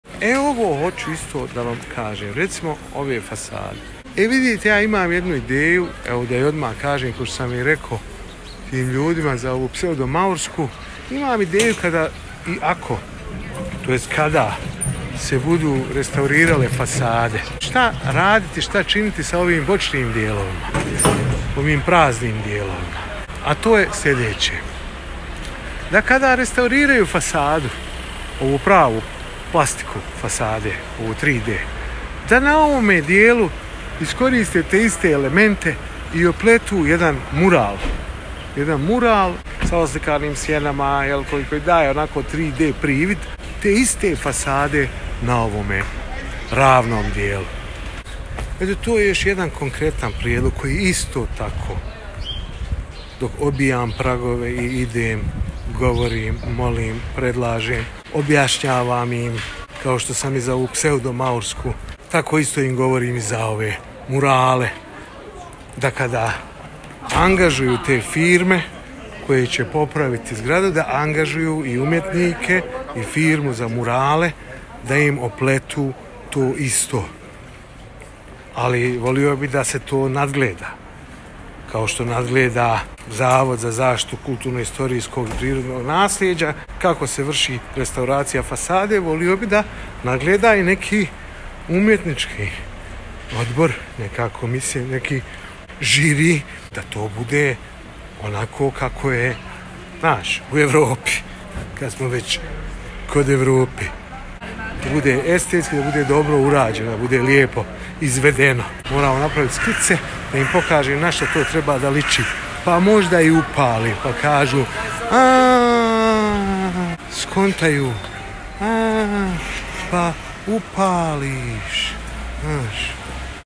UPUTSTVO za MURALE i FASADE
Kako tretirati fasadu u jednom glavnom gradu